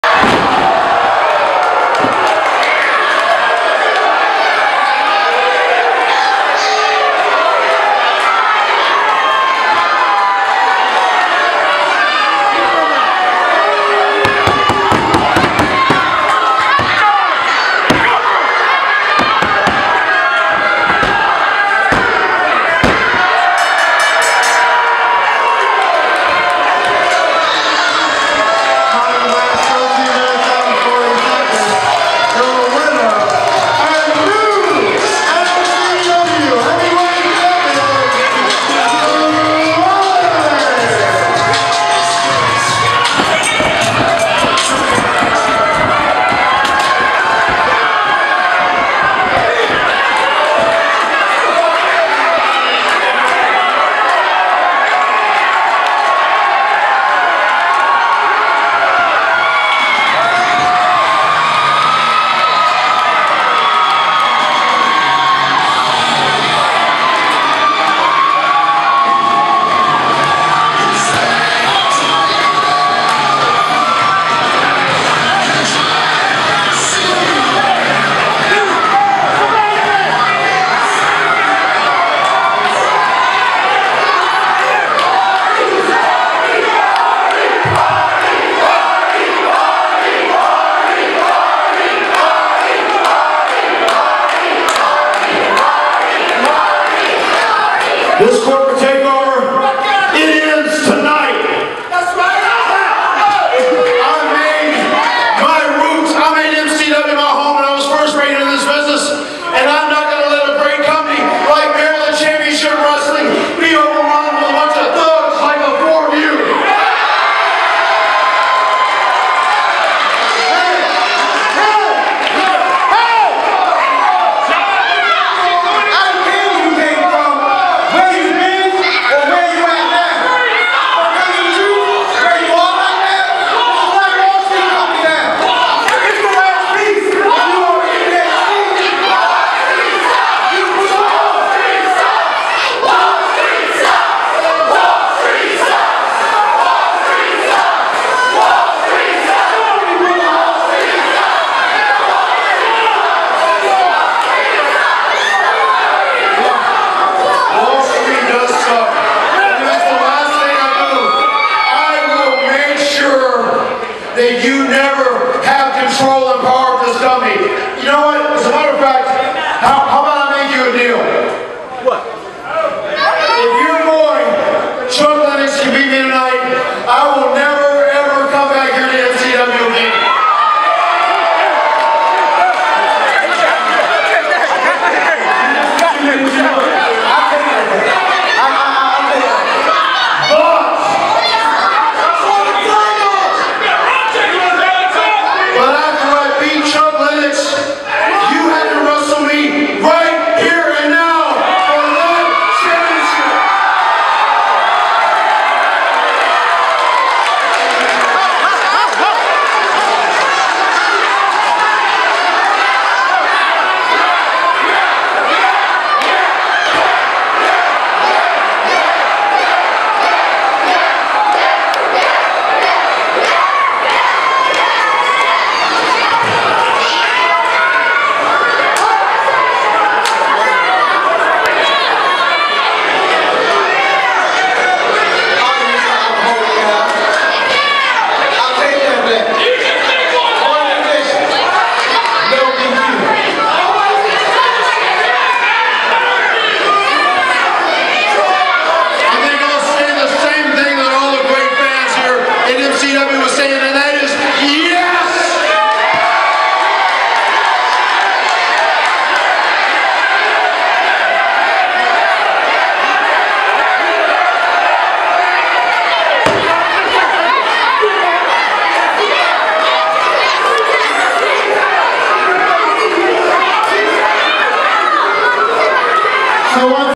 Drolix wins heavyweight Title 5 3 14
Maryland Championship Wrestling 5-3-14 Blk Wallstreet run in and cause Christian York to loose MCW Heavyweight Title and their brutal attack on York. Matt Hardy saves York and lays down the challenge to Drolix for later in the evening in Woldorf, MD.